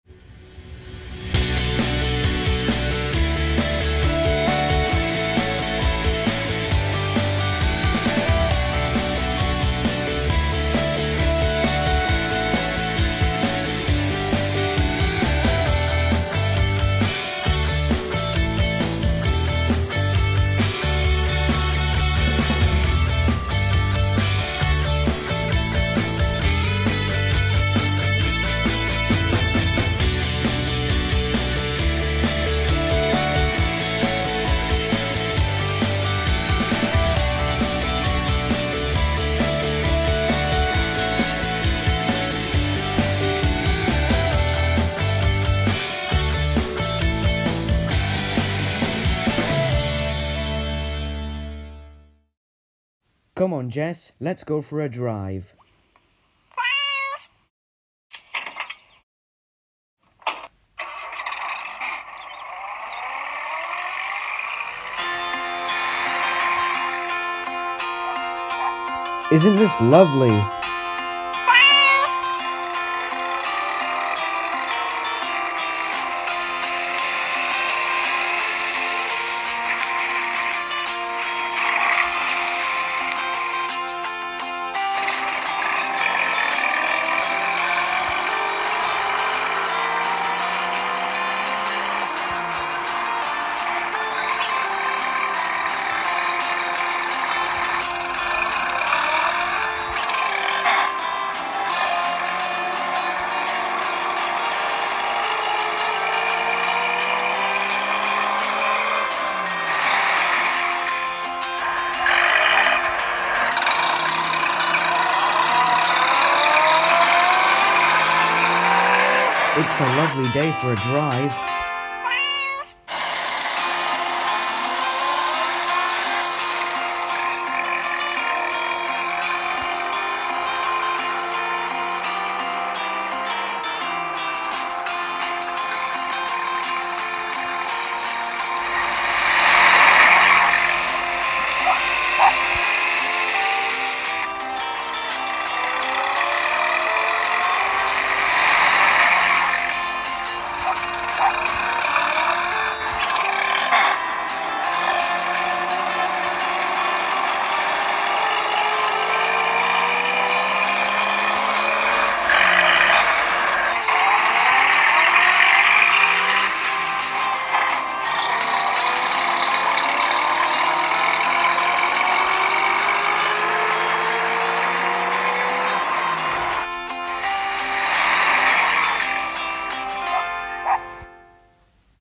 Postman Pat’s Big Drive
Since Pat and Jess have finished the post they decided to go for a drive in the van.